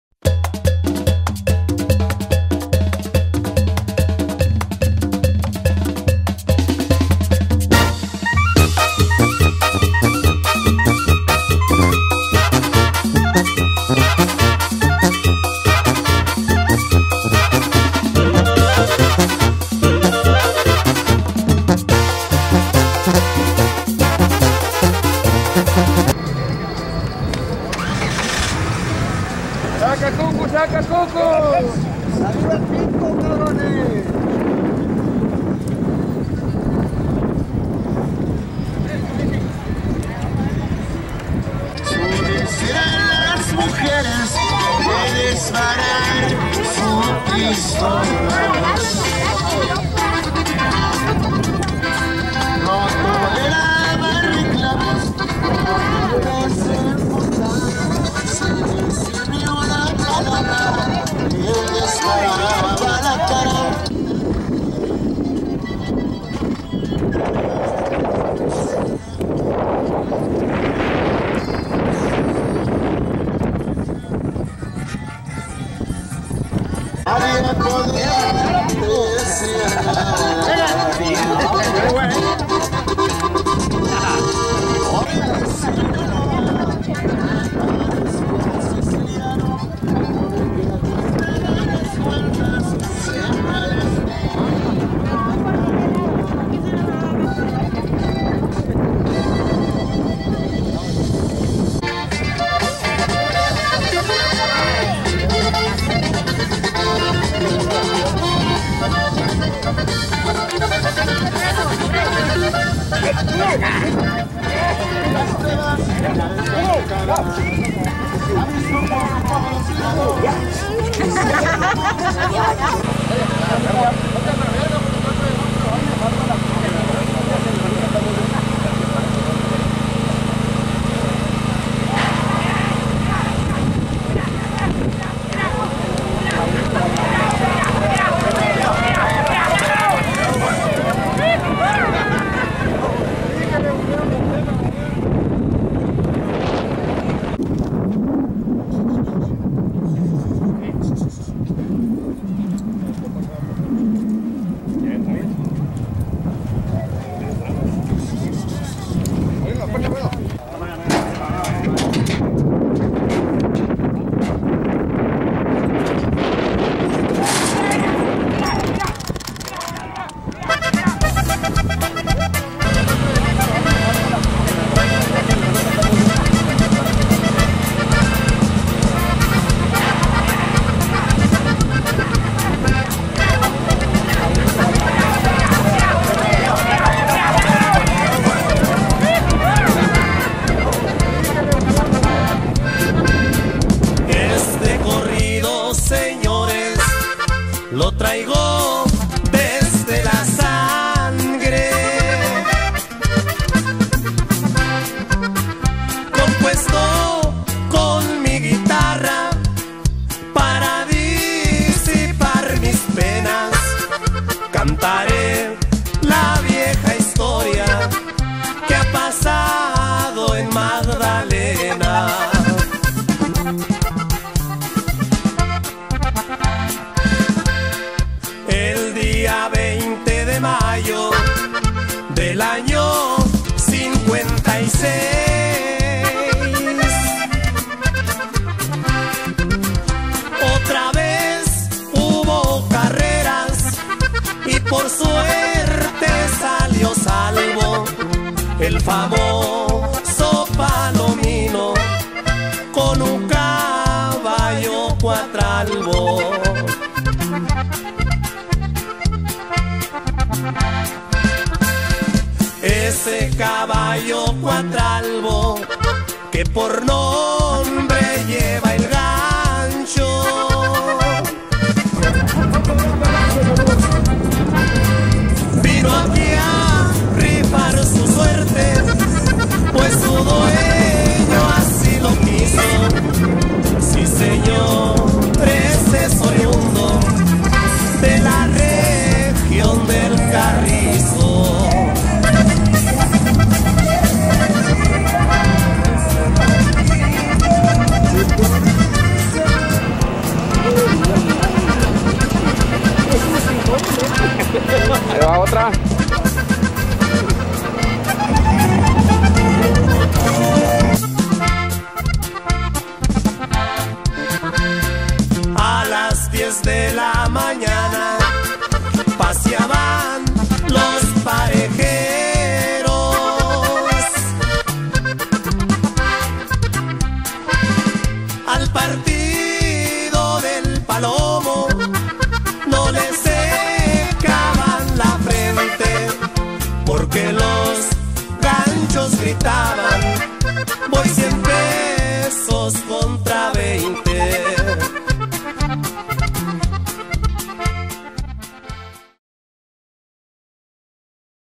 CARRERAS EN ESTACION LLANO 19.6.22 1A palomino vs cucu
1a carrera del sabado 22 de junio 2019.
Taste "Los Trejo" Estación Llano, mpio de Santa Ana, Son. Mex.